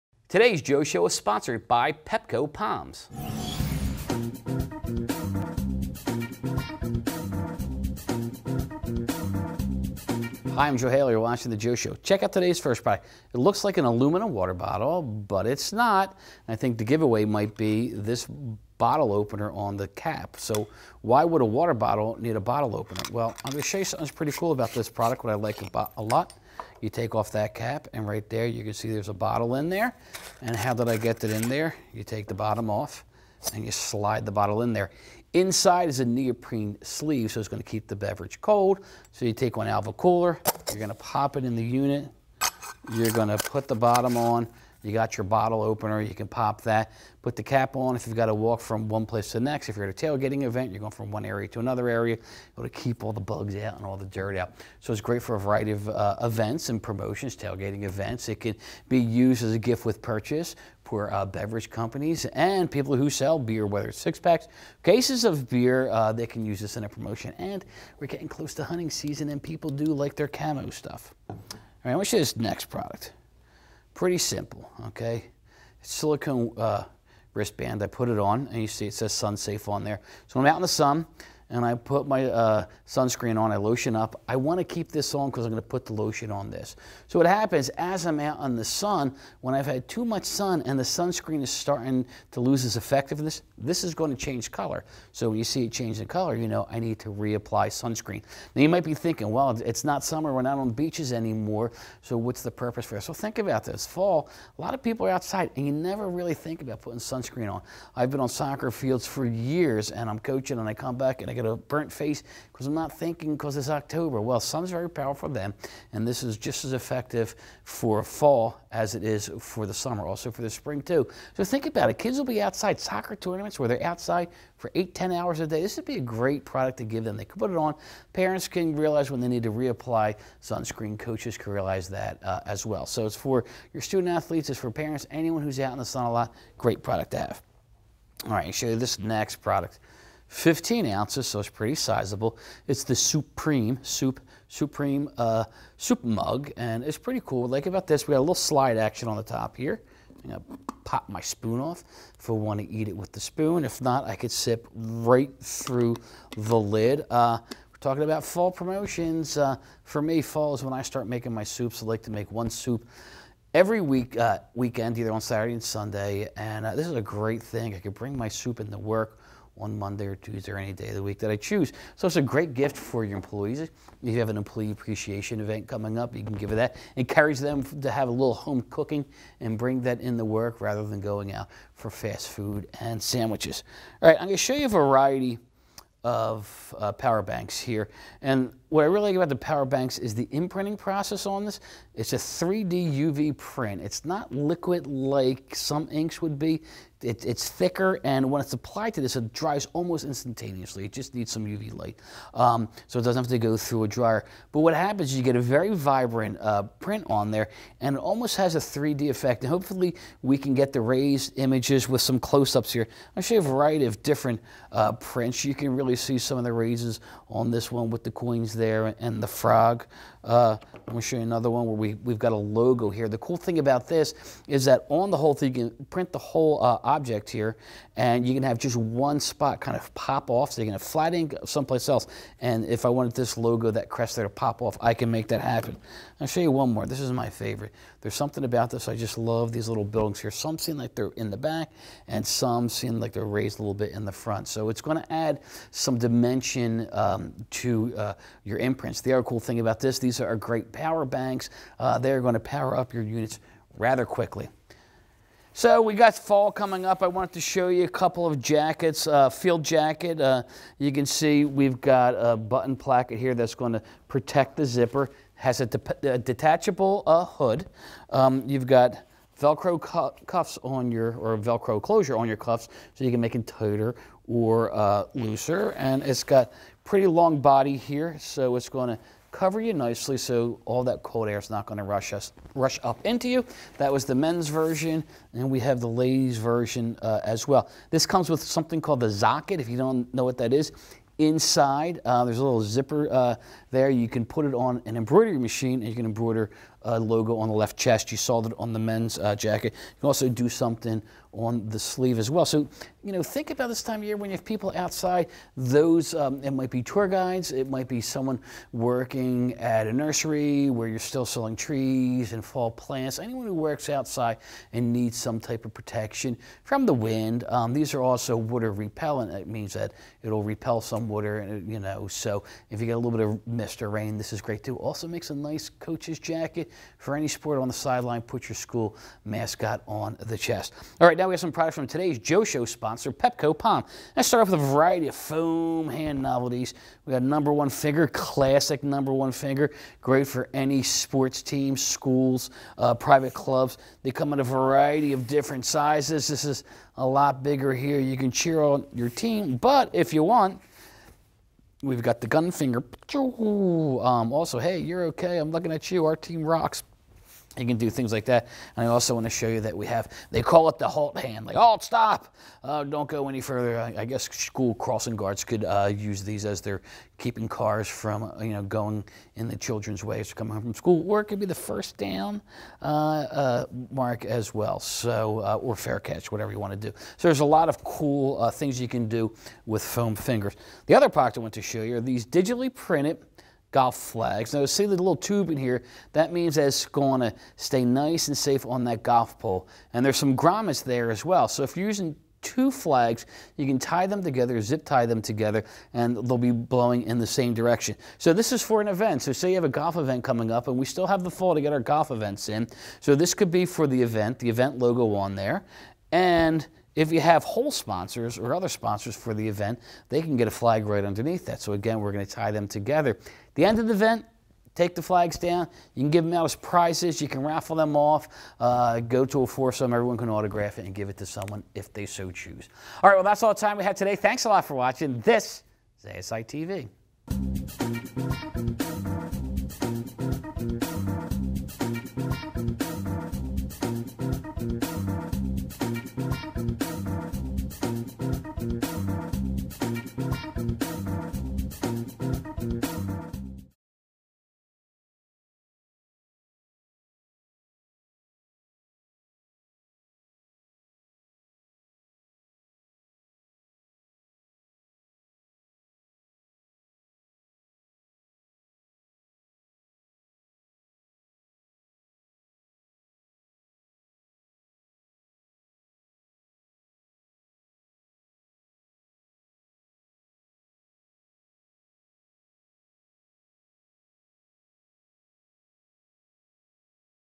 Powerful New Products - The Joe Show
In this episode of The Joe Show, Product Editor Joe Haley shows off an undercover bottle cooler masquerading as an aluminum water bottle. He also displays a bracelet that alerts users to reapply sun screen, a versatile fall field jacket and a variety of power banks with eye-catching color imprinting.